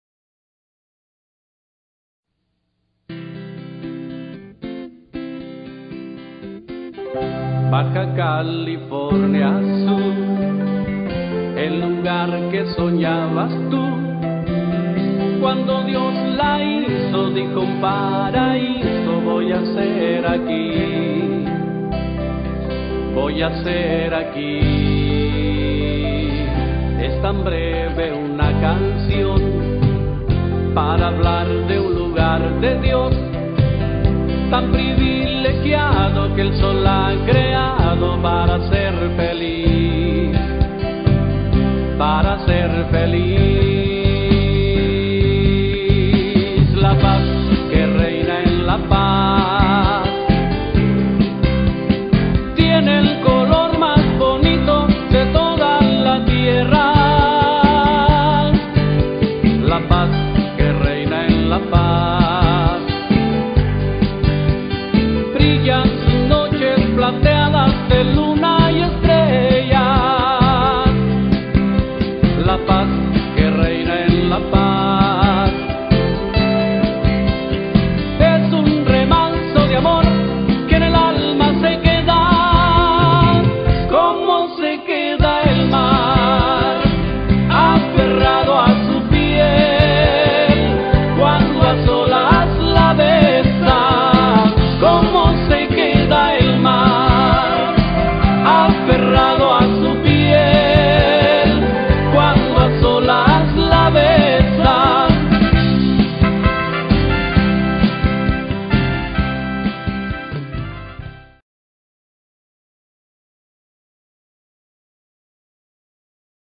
Caminata Unido por la paz & seguradad 4/1/15
El 04 de enero 2015 más de 10.000 personas de todos los ámbitos se reunieron en La Paz para caminar desde el kiosco del malecón hasta la explanada del palacio de gobierno para exigir a las autoridades estatales y federales que trabajen juntos para poner fin a la actual ola de violencia del narco que ha mostrado su lado oscuro en la Ciudad de la Paz